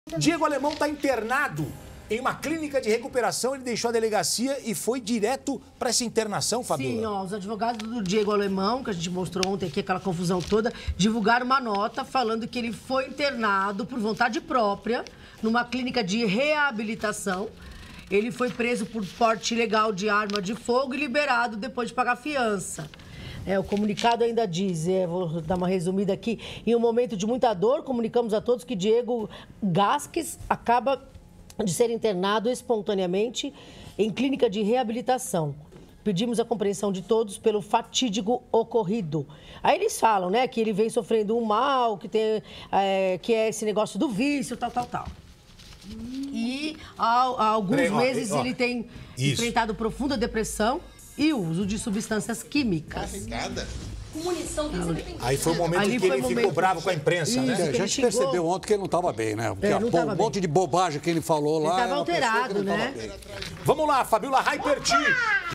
Diego Alemão se interna em clínica de reabilitação após sair da cadeia por porte ilegal de arma
O ex-participante de reality show se internou, por vontade própria, em uma clínica de reabilitação. Alemão havia sido preso por porte ilegal de arma de fogo e liberado depois de pagar fiança. De acordo com um comunicado, o ex-participante de reality show enfrenta uma depressão profunda e faz uso de substâncias químicas.